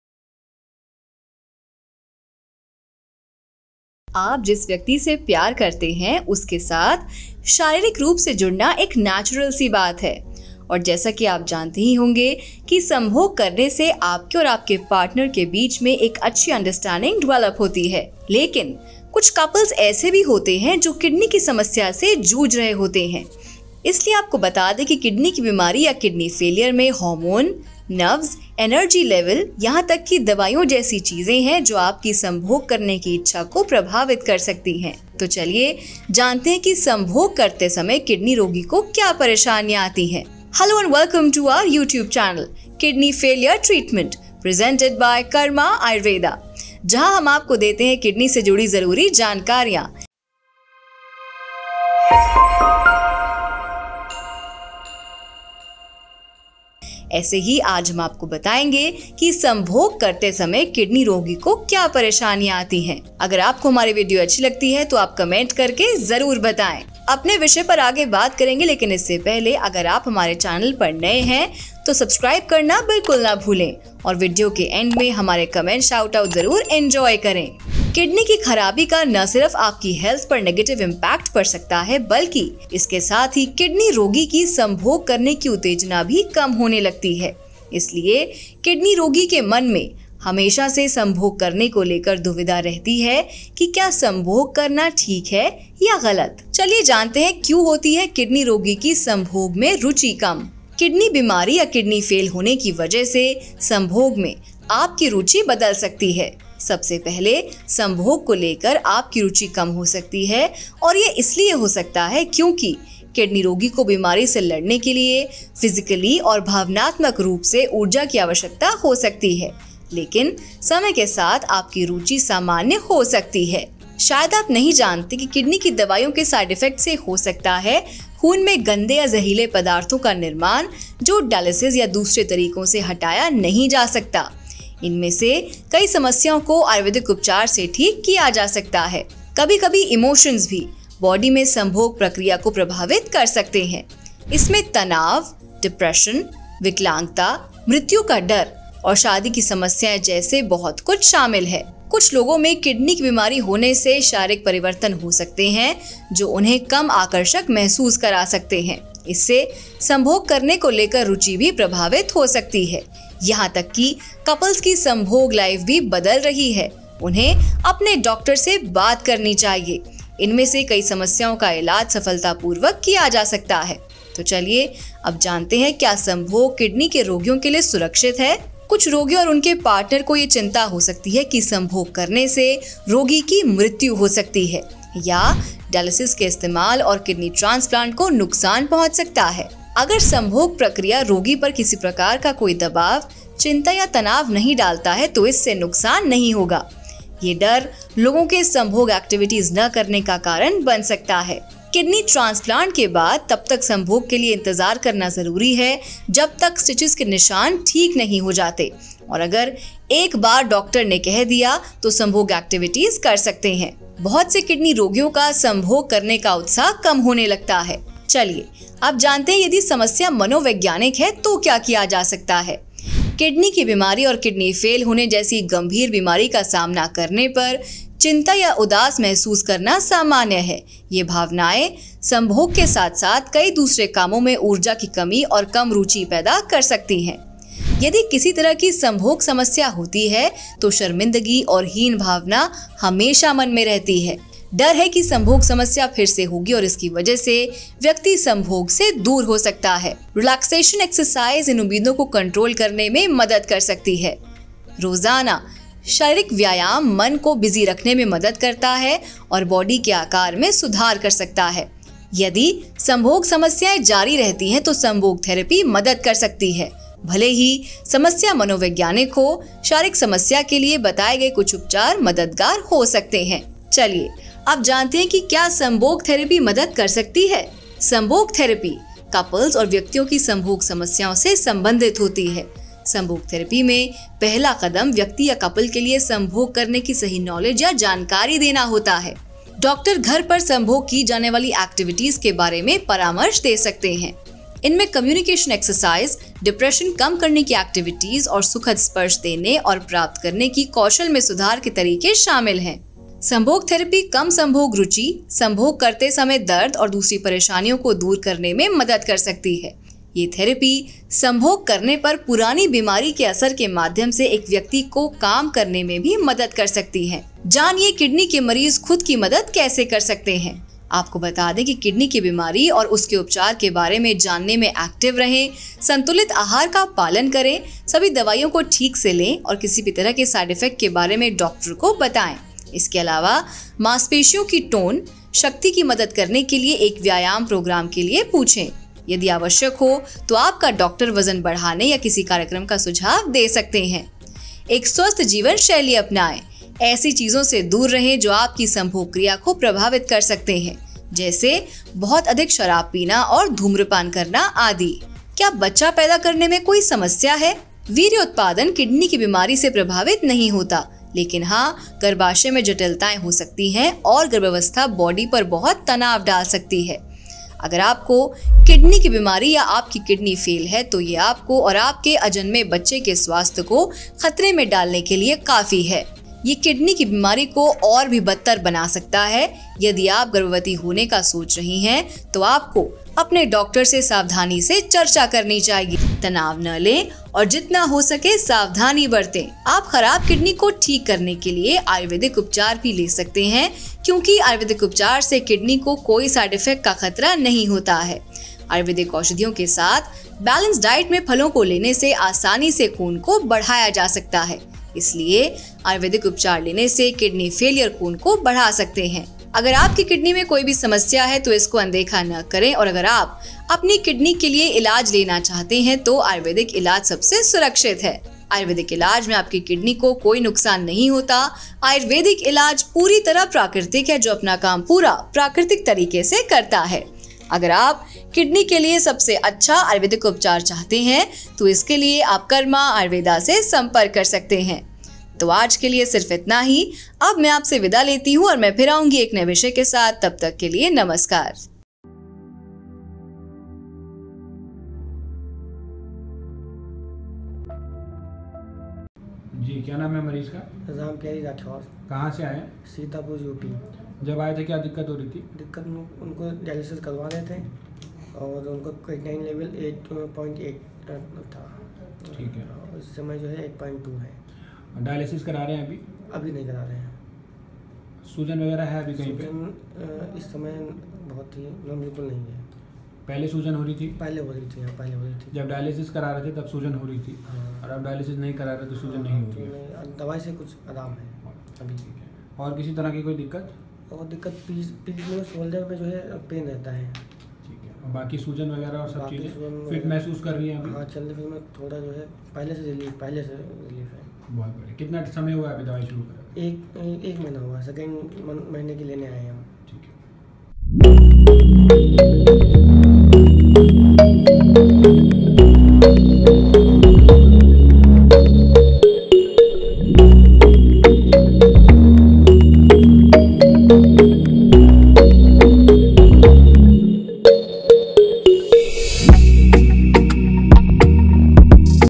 किडनी की बीमारी आपके यौन जीवन पर कैसे प्रभाव डालती है -Sexual dysfunction I Kidney failure treatment
You can also whats app us by clicking on this

किडनी रोग के लिए सर्वोत्तम आयुर्वेदिक उपचार पाने के लिए कृपया इस लिंक पर क्लिक करके हमें व्हाट्सएप करेंकिडनी की बीमारी आपके यौन जीवन पर कैसे प्रभाव डालती है -Sexual dysfunction I Kidney failure treatment

It is natural to be physically involved with the person you love, and you must know that having sex develops a good understanding between you and your partner.
But some couples are struggling with kidney problems. So let us tell you that in kidney disease or kidney failure, hormones, nerves, energy levels, and even medicine can affect your desire to have sex.
So let's know what problems a kidney patient faces while having sex?
Where we give you much important information related to kidneys.
If you like our video, then you must tell us by commenting.
We will talk further on our topic, but before that, if you are new to our channel, don't forget to subscribe to us and enjoy our Comment Shoutouts at the end of the video.
Kidney failure can not only harm your health, but along with it, the stimulation of sexual intercourse of the kidney patient also starts decreasing. Therefore, there is always a dilemma in the mind of a kidney patient about having sex, whether it is right or wrong to have sex?

Many kidney patients lose their enthusiasm for intercourse because they feel that intercourse is not as necessary as it was before. But activities like hugging and kissing are not involved in sexual intercourse but give rise to that feeling. For this, you can consult another sexual therapy from an intercourse specialist.

If there is an orgasm of any kind, embarrassment and inferiority complex are always on the mind. There is a fear that the intercourse problem will happen again, and because of this, the person may withdraw from intercourse. Relaxation exercises can help to control these expectations.

Daily physical exercise helps to keep the mind busy and can improve body shape. If orgasmic problems persist, orgasm therapy may help; even if the problem is psychological, some of the treatments prescribed for physical problems may be helpful.

Let us now know whether intercourse therapy can help?
Intercourse therapy deals with the intercourse problems of couples and individuals. The first step in intercourse therapy is to provide the correct knowledge or information for the person or couple to have intercourse. The doctor can advise about sexual intercourse activities at home.

These include communication exercises, depression-reducing activities, and ways to improve the skills of giving and receiving pleasant touch.
Orgasm therapy can help relieve low sexual interest, problems reaching orgasm, pain during intercourse, and other problems. This therapy can also help a person work through the effects of a chronic illness upon having intercourse.

Know how kidney patients can help themselves.
Let us tell you that be proactive in knowing about kidney disease and its treatment, follow a balanced diet, take all medicines properly and tell the doctor about any kind of side effects; apart from this, muscle tone, Ask for an exercise program to help power-up.
Your doctor may suggest weight gain or a program if necessary. Adopt a healthy lifestyle.

Are there any problems with having children?
Sperm, i.e., semen production, is not affected by kidney disease, but yes, there can be complications in the uterus, and pregnancy can put a lot of stress on the body.
If you have kidney disease or kidney failure, this is enough to put you and your unborn baby's health at risk.

Now you must understand that kidney disease does not affect sexual life like this, but if you adopt a lifestyle like normal people, then you may have problems, so there is a need to be aware. Don't stress and be careful as much as you can.

You can also take ayurvedic treatment to cure a bad kidney as there is no risk of any side-effect to the kidney with ayurvedic treatment.

If there is any problem in your kidney, then do not ignore it, and if you want to take treatment for your kidney, then Ayurvedic treatment is the safest.

Because in ayurvedic treatment, there is no harm to your kidney in any way.

If you want the best Ayurvedic treatment for kidneys, then you can contact Karma Ayurveda for this.
So that's all for today.